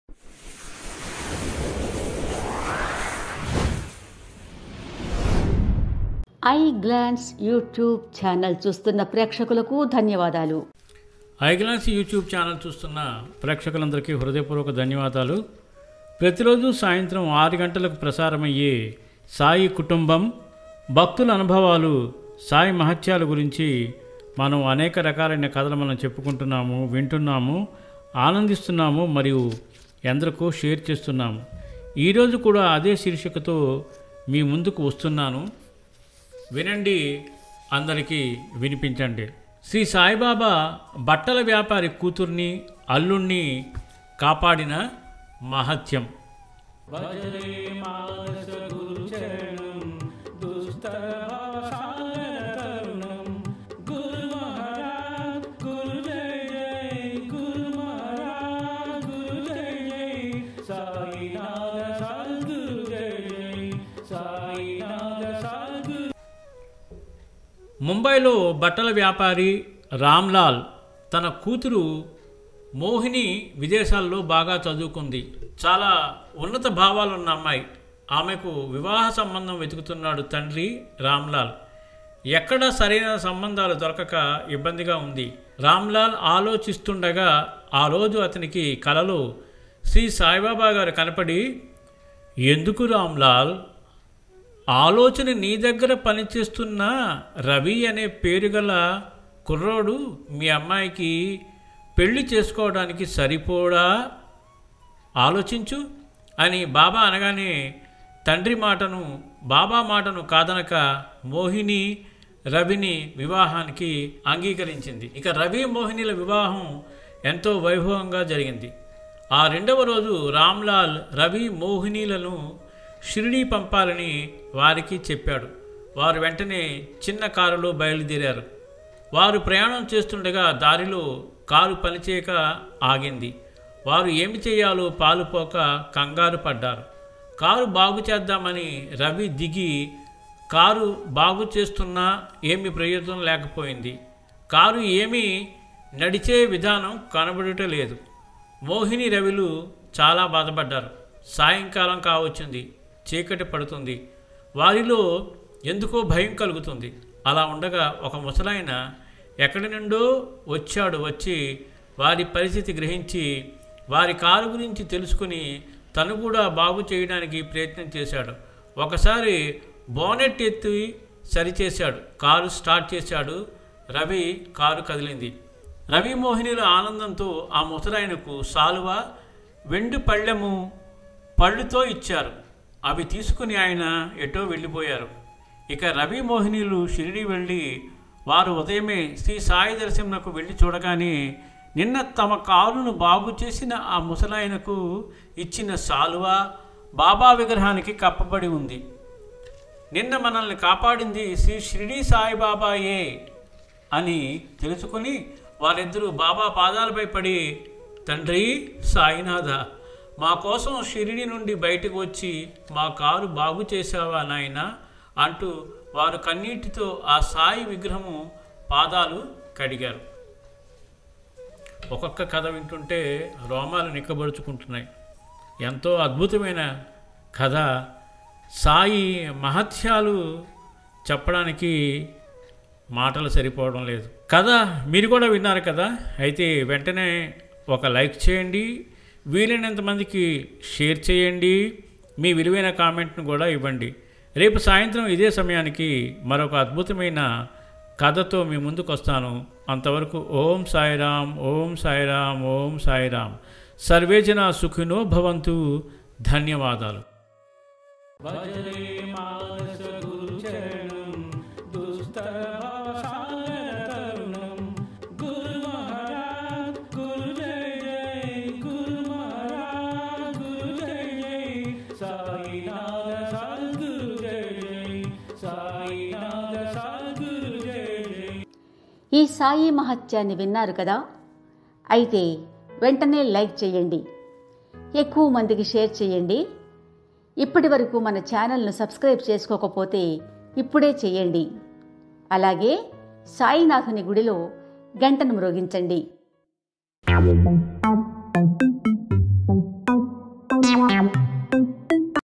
బాబా బట్టల వ్యాపారి కూతుర్ని,అల్లుడ్ని కాపాడిన మహత్యం||సాయిమహత్యం 88|| సాయి కుటుంబం||eye glance
eye glance
వ్యాఖ్యానం: అక్కునూరి సీతారమ మోహనరావు